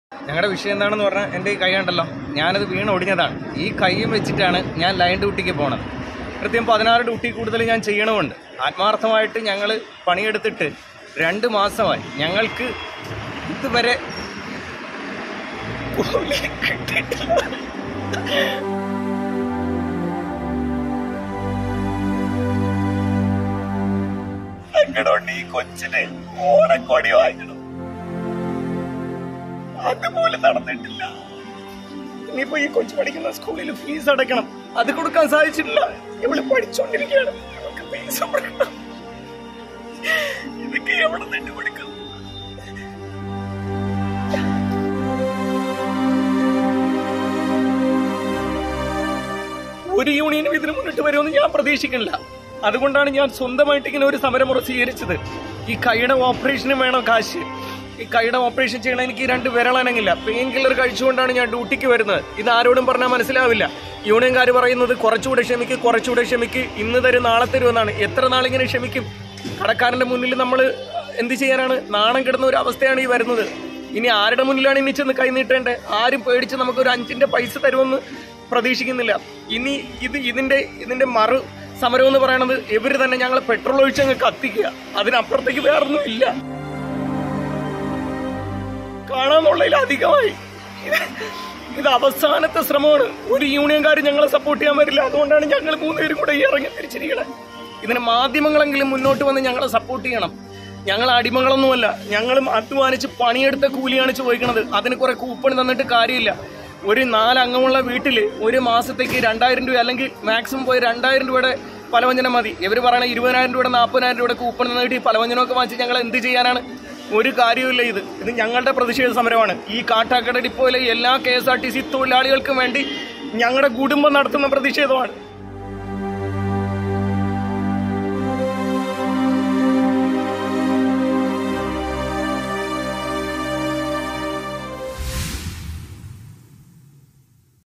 KSRTC SALARY CRISIS; കാട്ടാക്കട ഡിപ്പോയിൽ ജീവനക്കാരനും കുടുംബവും നിൽപ് സമരത്തിൽ | KSRTC EMPLOYEE
കെഎസ്‌ആർടിസിയിലെ ശമ്പള പ്രതിസന്ധിയുടെ പശ്ചാത്തലത്തിൽ തിരുവനന്തപുരം കാട്ടാക്കട ഡിപ്പോയിൽ ജീവനക്കാരന്‍റെ പ്രതിഷേധം. കണ്ടക്‌ടർ ഗോപീഷ് ആണ് കുടുംബസമേതം നിൽപ് സമരം നടത്തി പ്രതിഷേധിക്കുന്നത്. രോഗബാധിതനായ താൻ ആത്മാർഥമായി ജോലി ചെയ്‌തിട്ടും രണ്ട് മാസത്തെ ശമ്പളം ഇതുവരെ ലഭിച്ചില്ലെന്ന് ഗോപീഷ് പറയുന്നു. ശമ്പളത്തിന് പകരം കൂപ്പൺ നൽകുന്നത് അംഗീകരിക്കാനാകില്ലെന്നും ഗോപീഷ്. |
KSRTC SALARY CRISIS EMPLOYEE PROTEST AT KATTAKADA BUS DEPOT